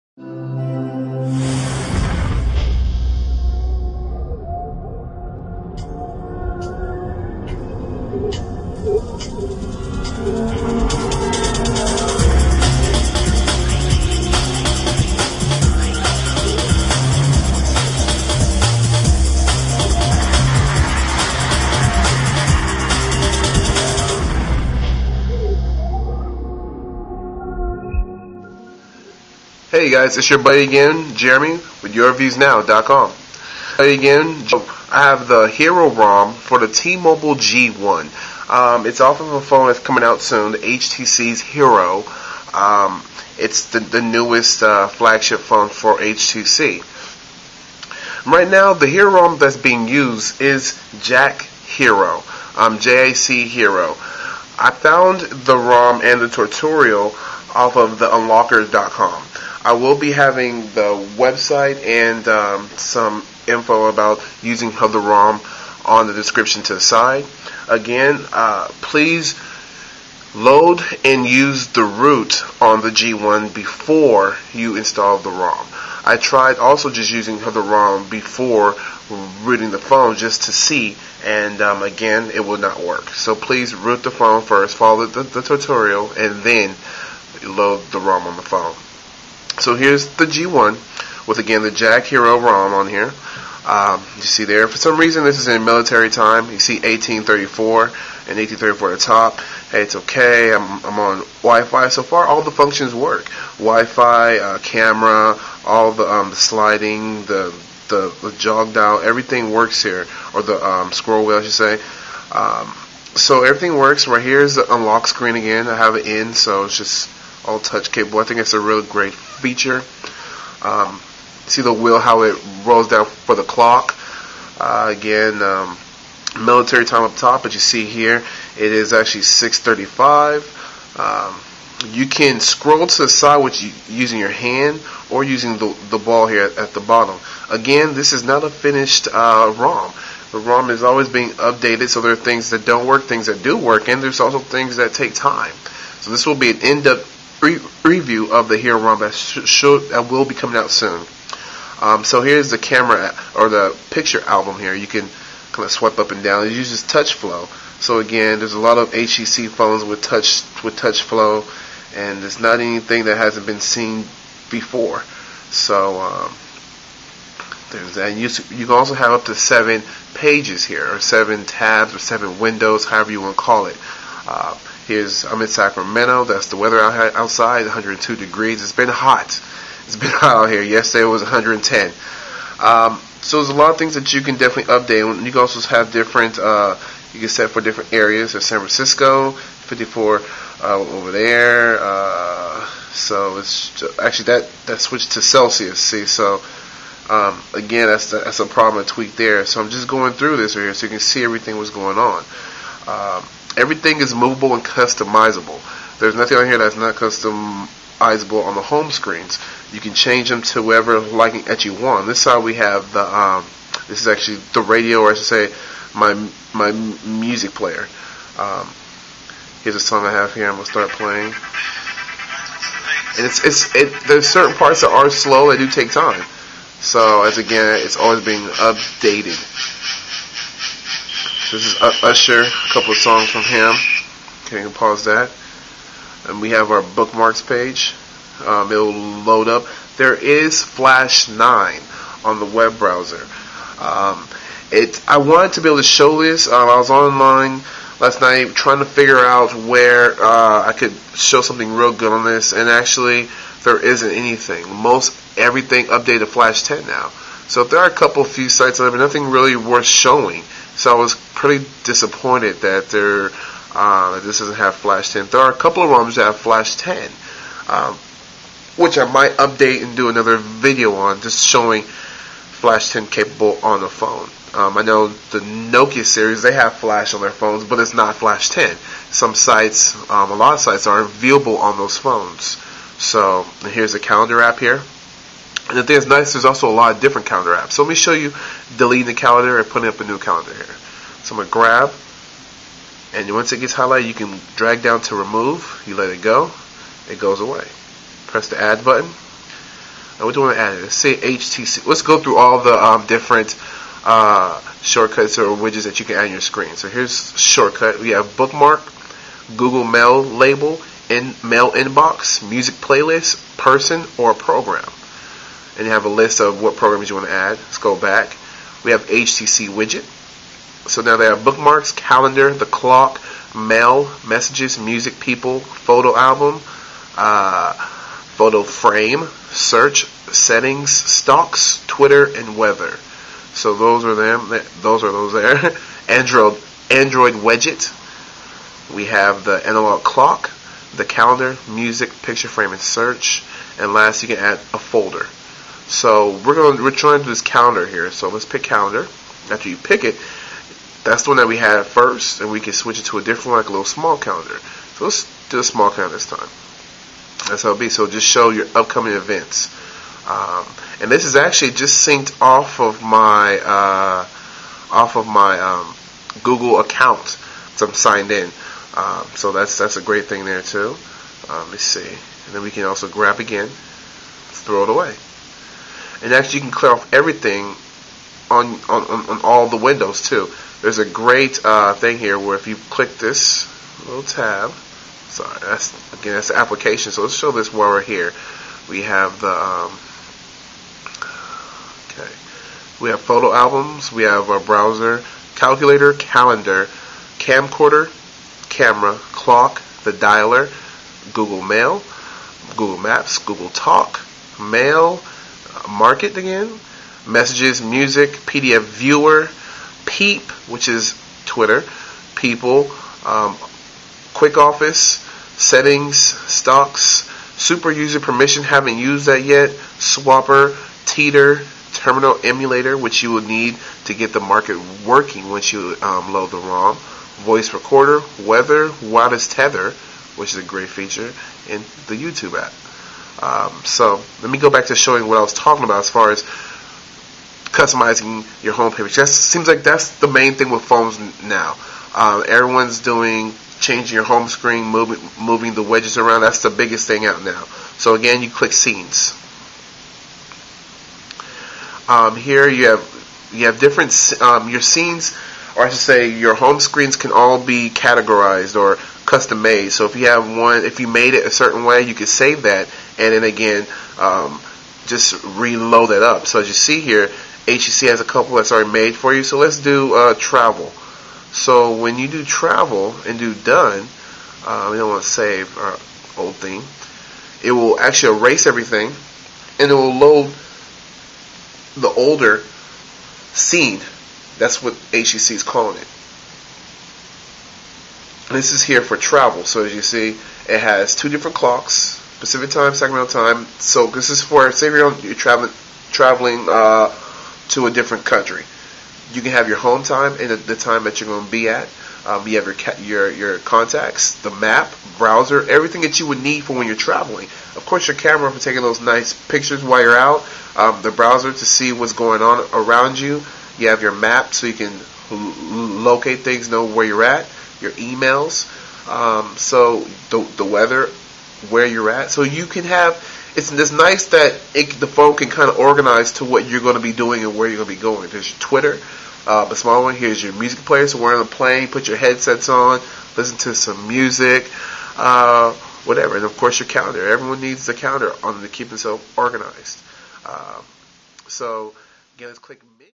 (PART 1)IN- DEPTH T-MOBILE G1 Review (HERO ROM) Plus Steps to load Hero Rom
(Part 1)
Jeremy doing a In-Depth review of T-mobiles G1 with the famous Hero Rom!!!